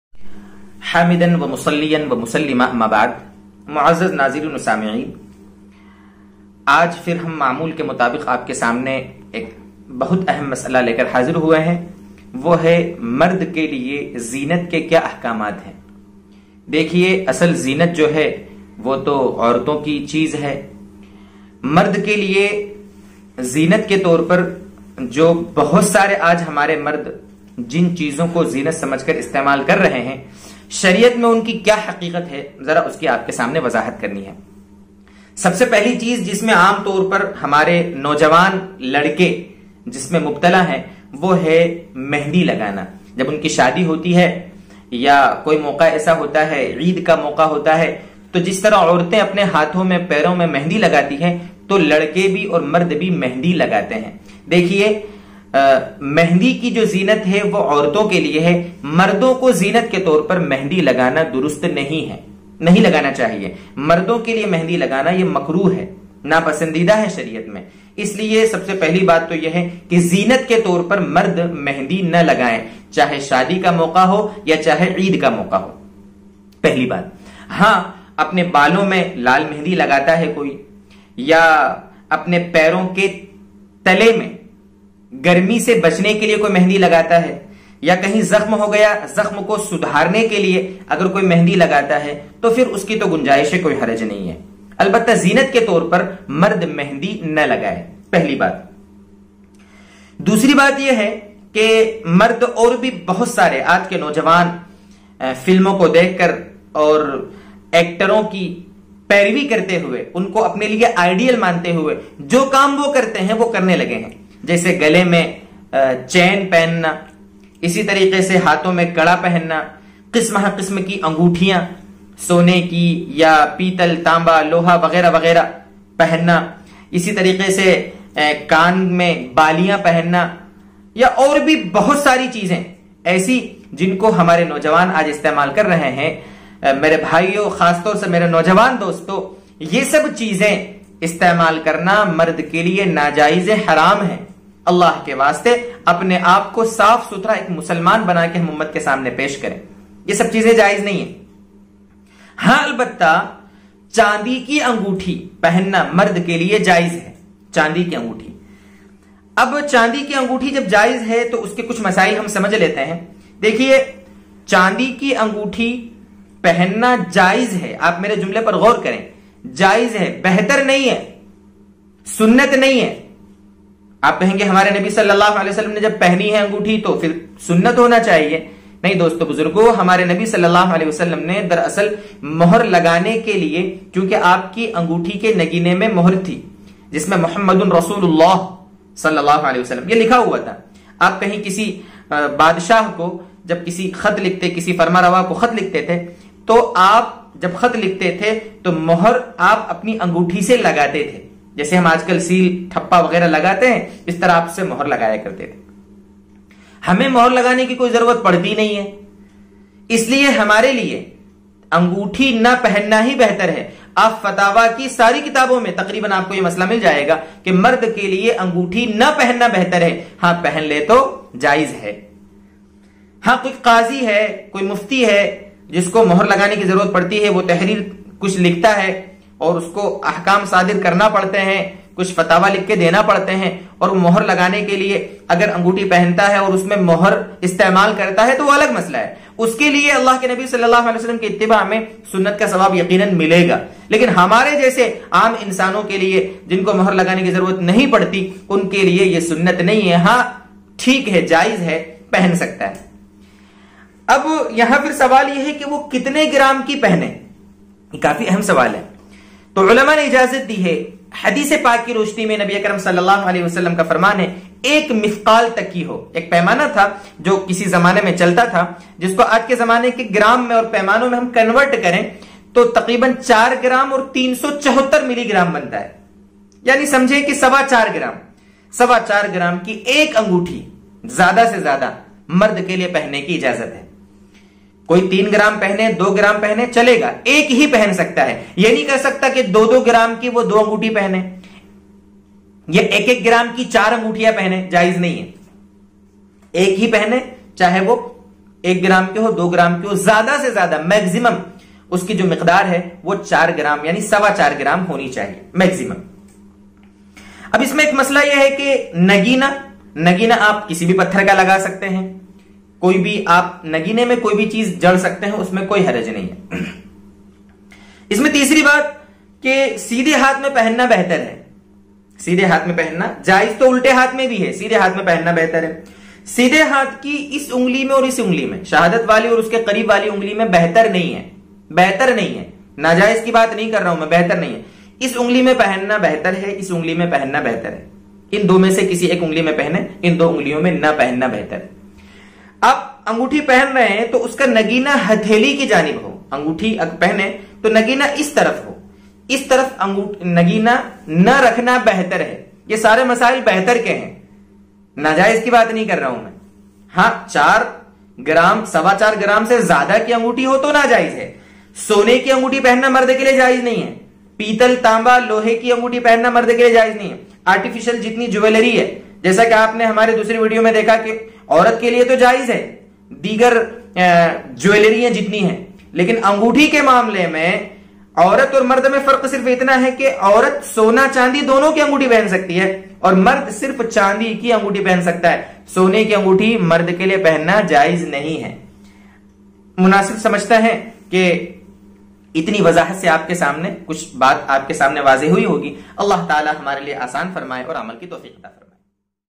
Mard Kaisi Anguthi Pehan Sakta Hai | Anguthi Pehanne Ke Masail | Mufti Idrees Falahi
Assalamu alaikum
Is video me mardon ke liye anguthi pehanne aur mehandi lagane isi tarah kade , baali aur chain pehanne ke baare me kuch aham aur zaruri masail batane ki koshish ki gayi hai...

Khud bhi sunen aur friends ko bhi share karen...

Mufti Idrees falahi official YouTube channel